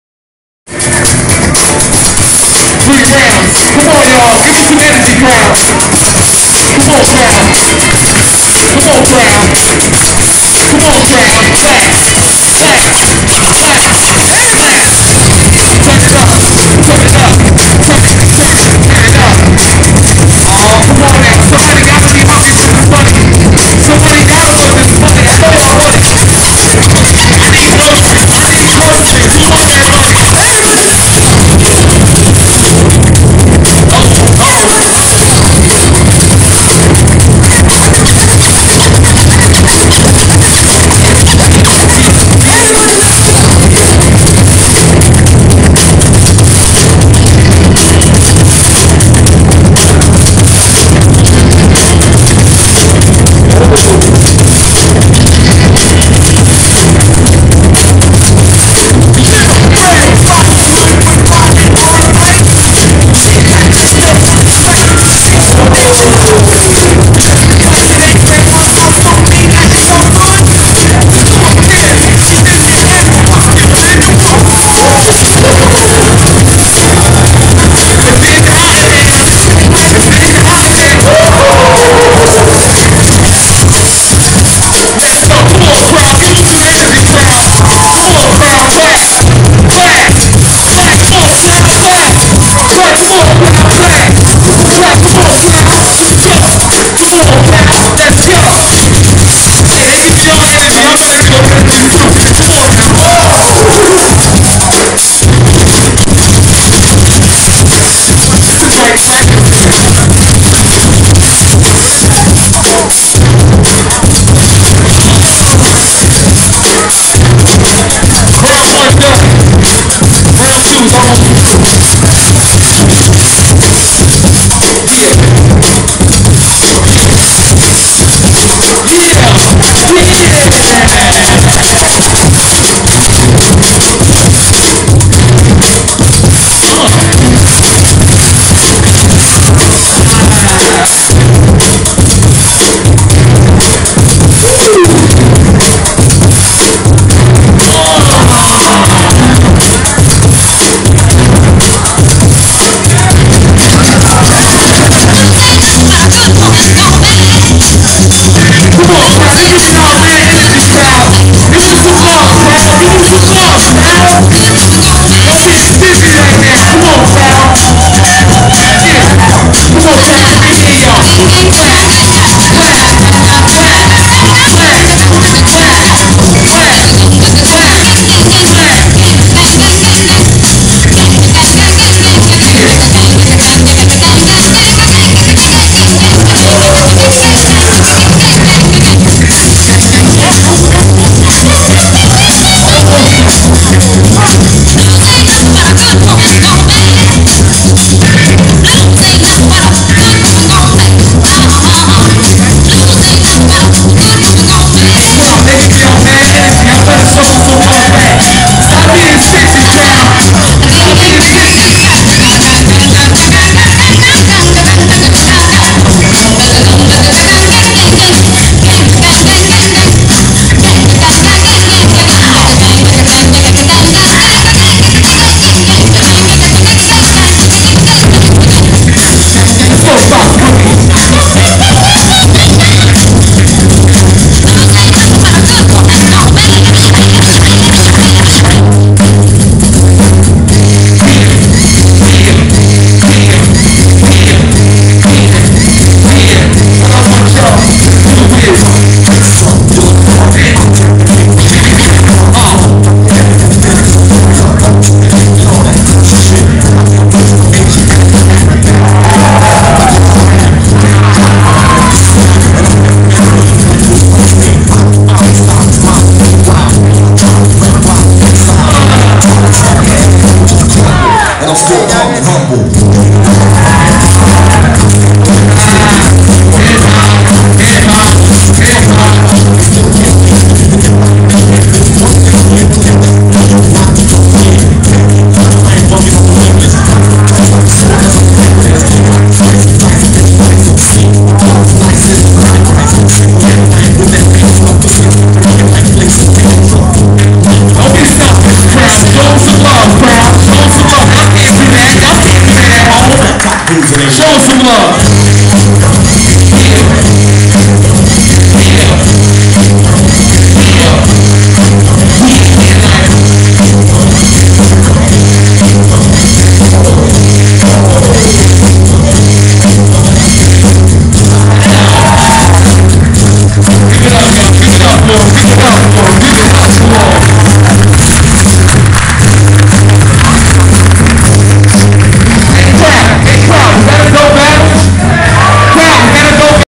The Gathering | King Of The Ring | 360 Legion Vs 360 Flava
360 Flava - Left
1st out - Bboy Super Josh
2nd - Bboy Ill Phil

360 Legion
1st out Bboy Ill Roc
2nd - Bboy Kev Roc